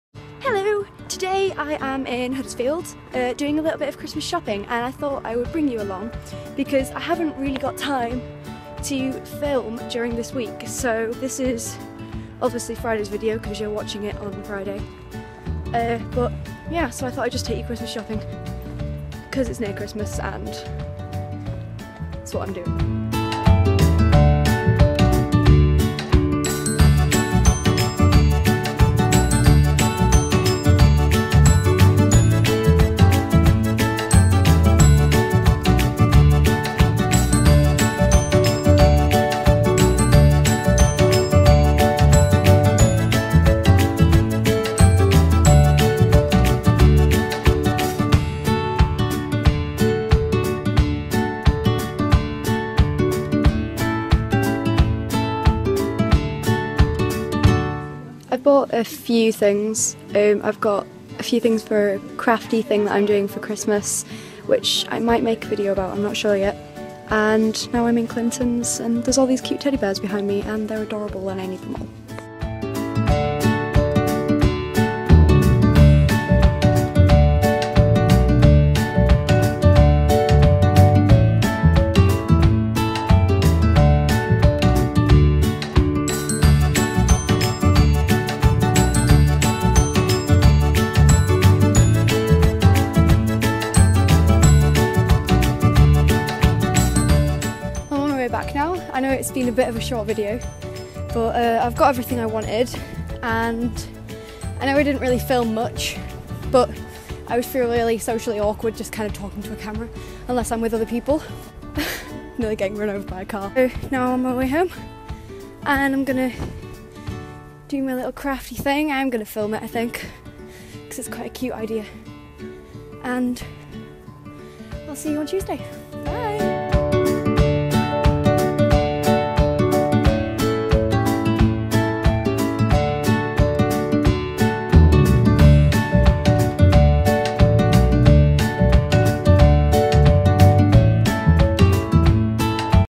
Christmas shopping!
I went christmas shopping in Huddersfield! Feel free to join me...